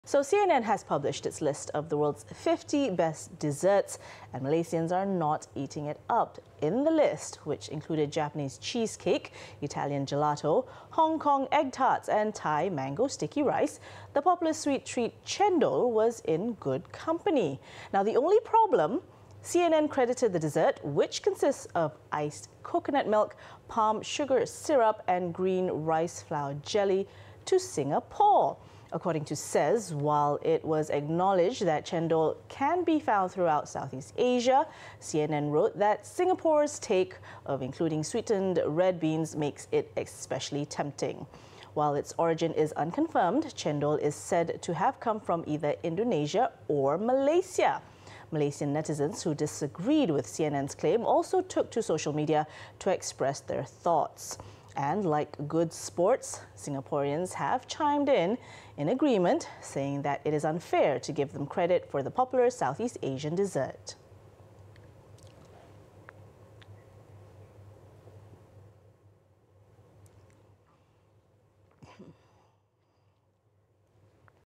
Clickbait: M'sians Up In Arms Over CNN Calling Cendol 'Singaporean'
CNN has published its list of the world's '50 Best Desserts' and Malaysians are not eating it up.

In the list which included Japanese cheesecake, Italian gelato, Hong Kong egg tarts, and Thai mango sticky rice, the popular sweet treat cendol was in good company.

The only problem? CNN credited the dessert which consists of iced coconut milk, palm sugar syrup, and green rice-flour jelly to Singapore.